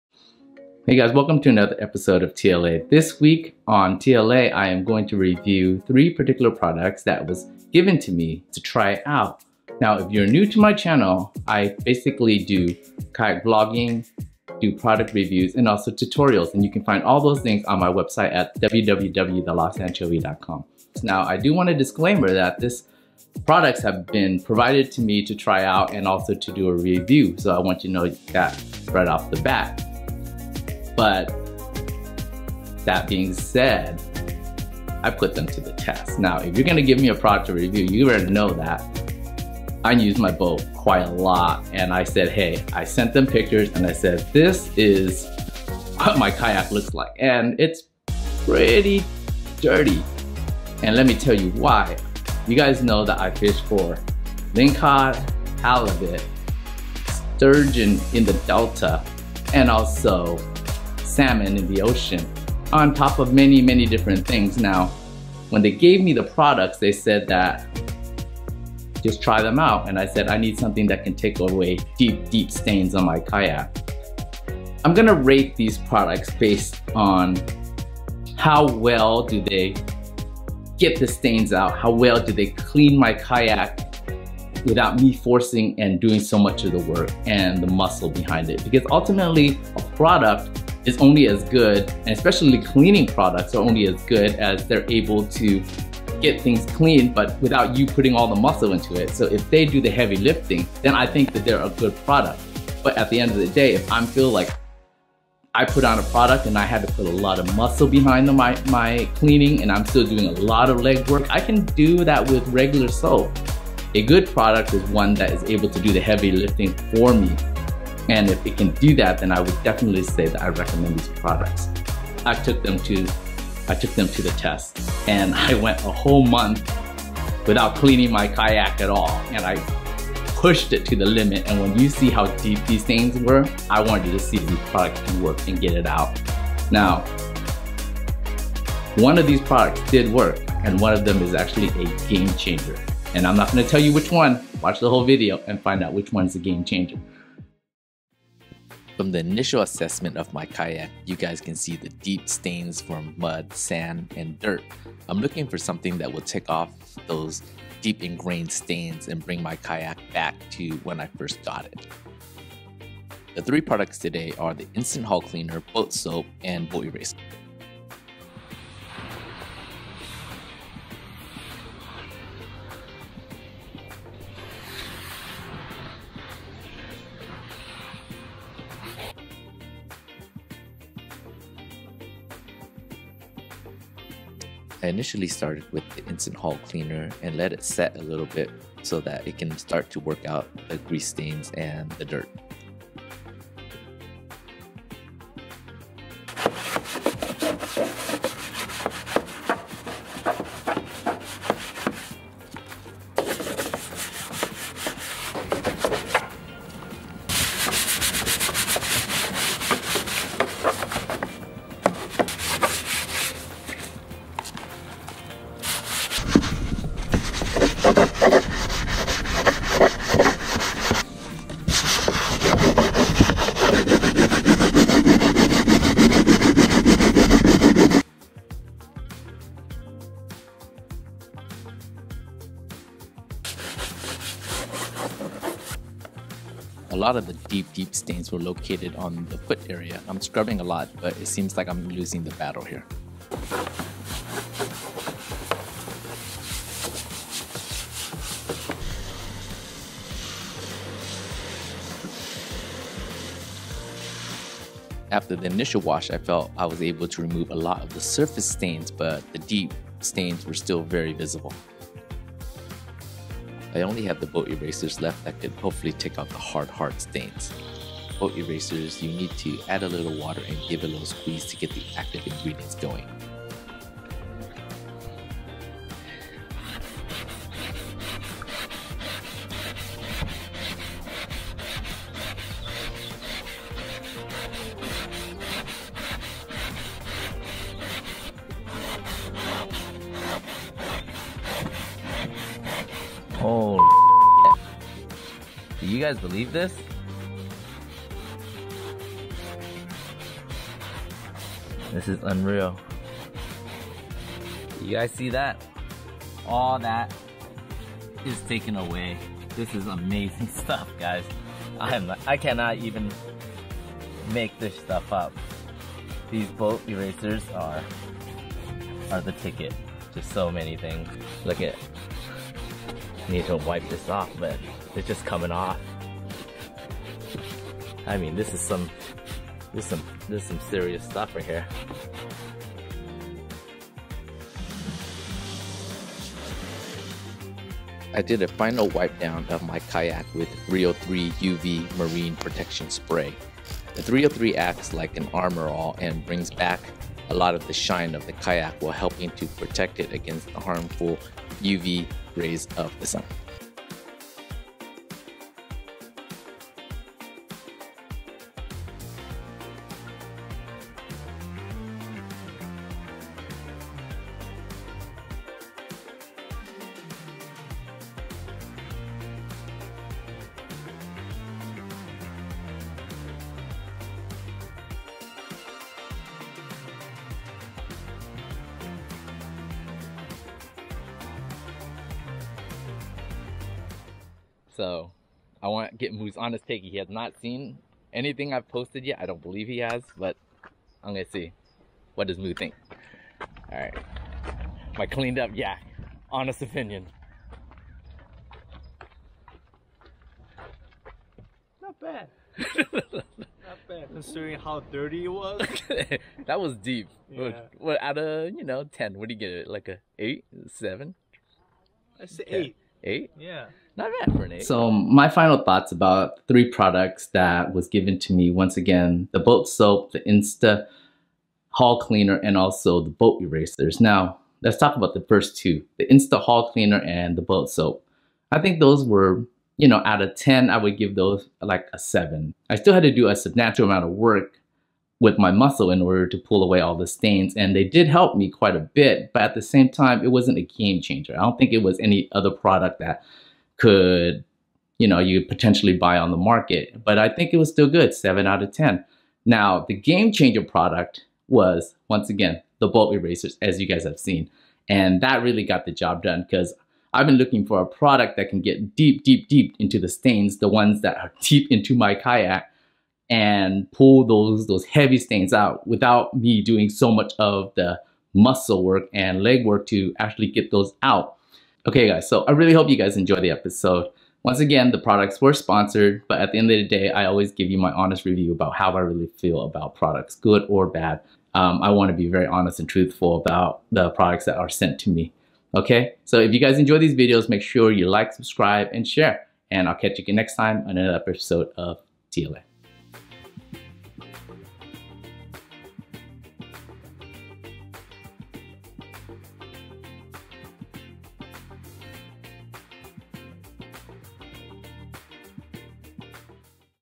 Product Review | Boat Erasers, Soap and Hull Cleaner ** ONE is a GAME CHANGER!
This week on TLA, I review three boat cleaning products that was sponsored to me. I didn't wash my kayak for a month in anticipation of this video. I had squid ink, mud, sand, lingcod and halibut blood and god knows what else on my kayak hull.

I put these products to the test, and GOD help the manufacturer of these products. I didn't expect much but FOUND one of these products is an ABSOLUTE GAME CHANGER! Watch the video this week to find out which one blew my mind!

Product Links!

Boat Erasers
Boat Soap
Hull Cleaner